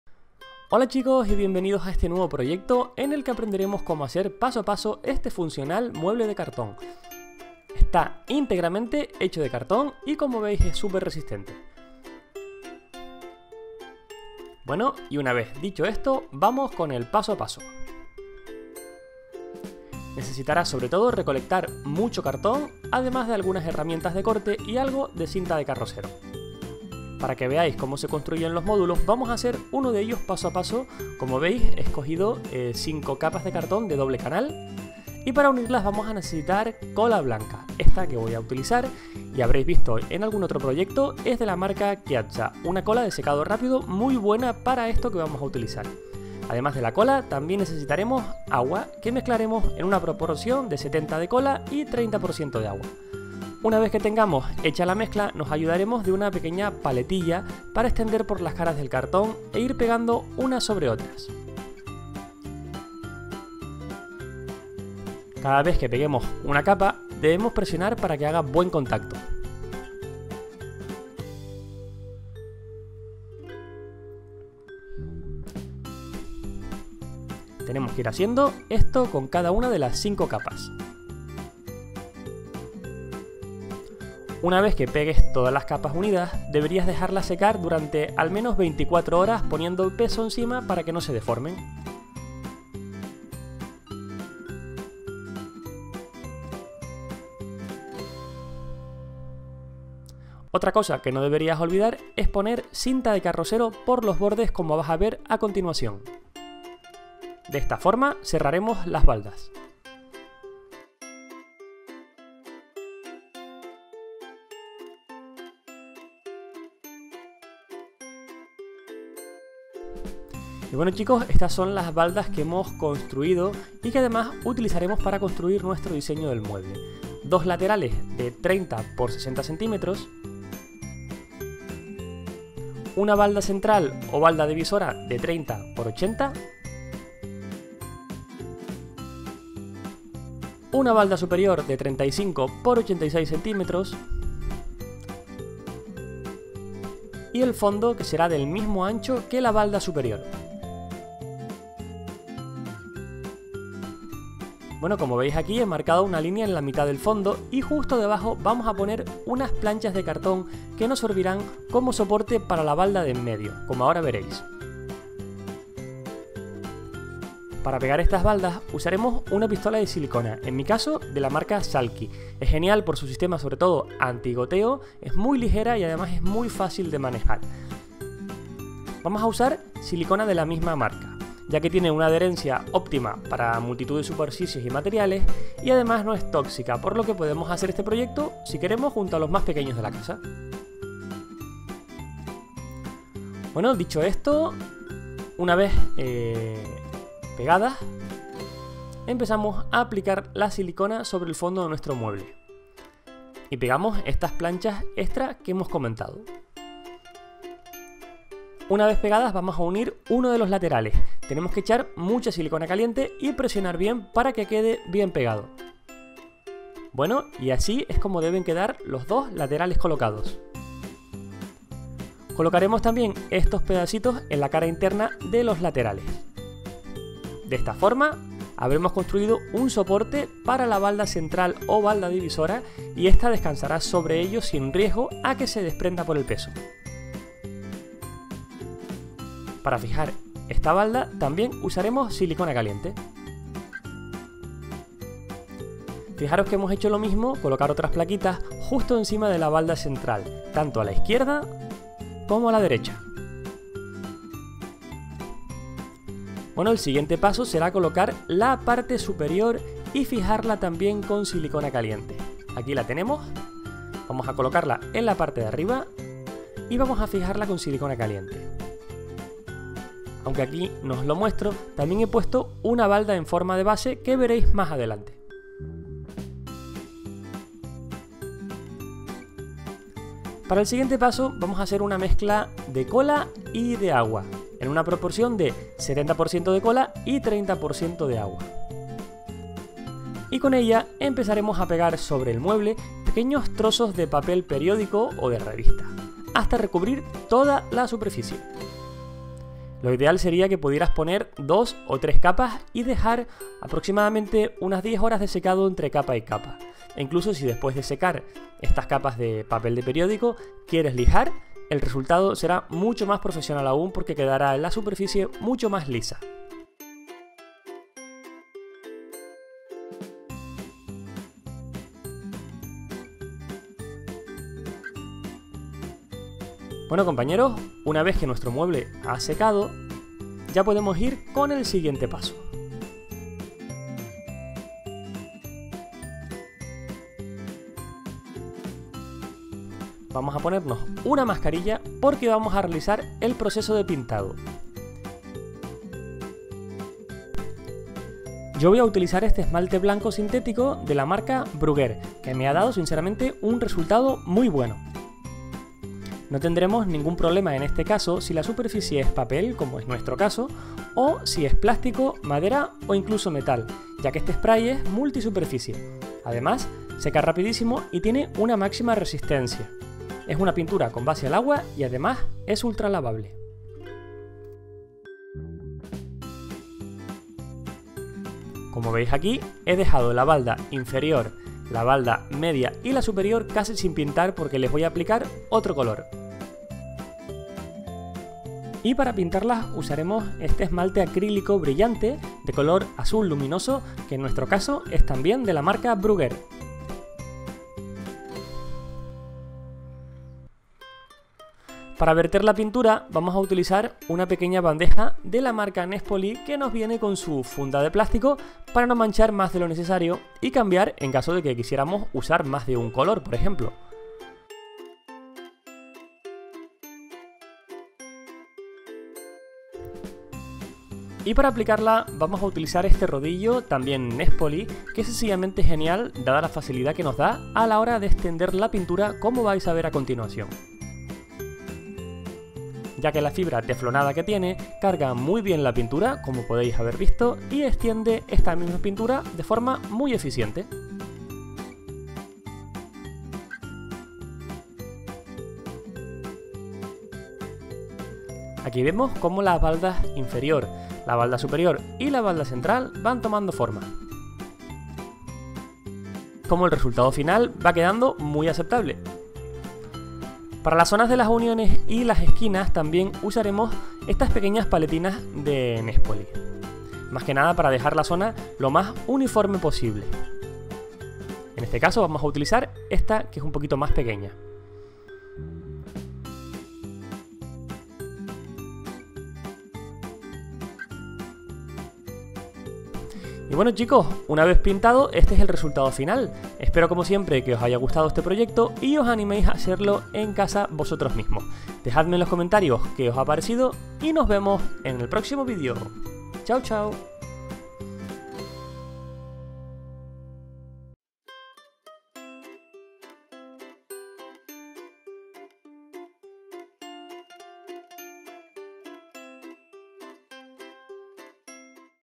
Como Hacer Un Mueble de Cartón | Manualidades con Reciclaje
Cómo Hacer un Mueble de cartón paso a paso y muy fácil. Un bonito Mueble usando materiales reciclados que puedes encontrar en casa.

Una forma sencilla de hacer manualidades, sin gastar dinero, y muy prácticas. Este mueble aguanta bastante peso, y es muy resistente. Anímate a realizar este proyecto!